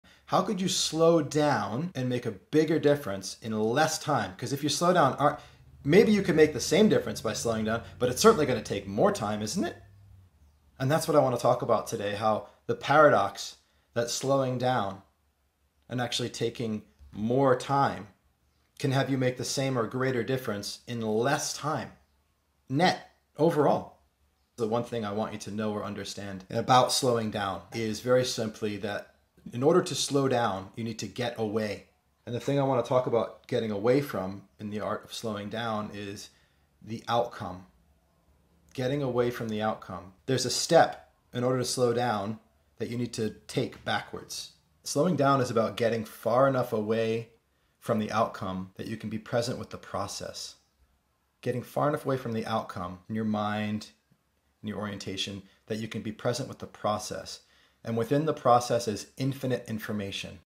How to Slow down and Make a Bigger Difference
What do you need to get away from in order to slow down and make a bigger difference in less time?

Loving you, JPM